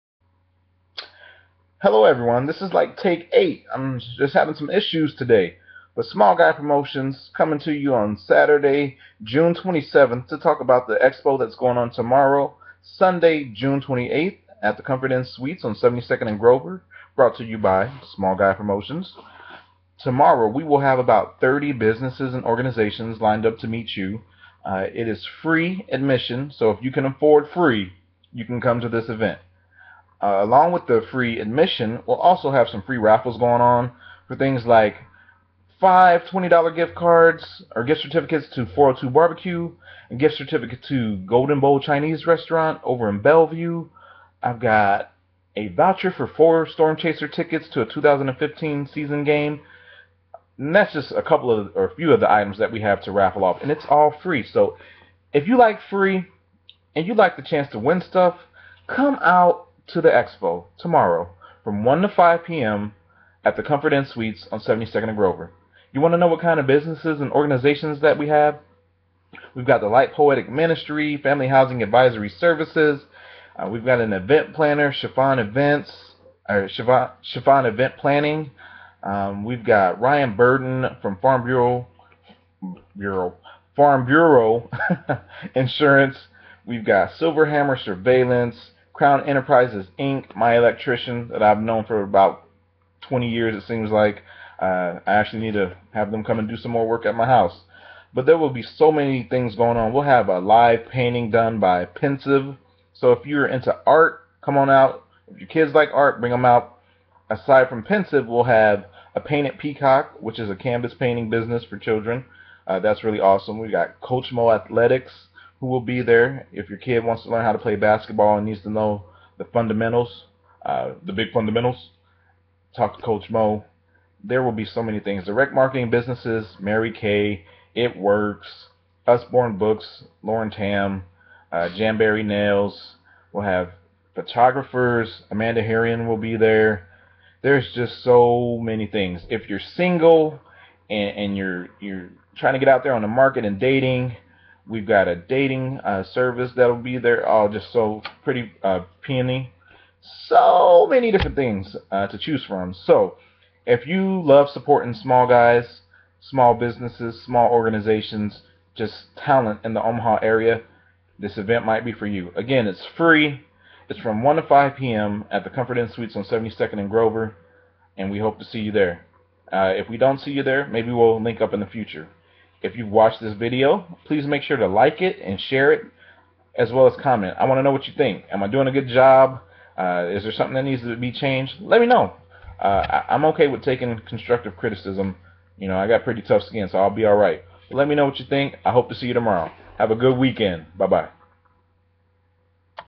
Expo on 6-28-15
This is a small promo video for our next expo tomorrow at the Comfort Inn Suites on 72 & Grover. People of Omaha, please come out and support all of these awesome organizations.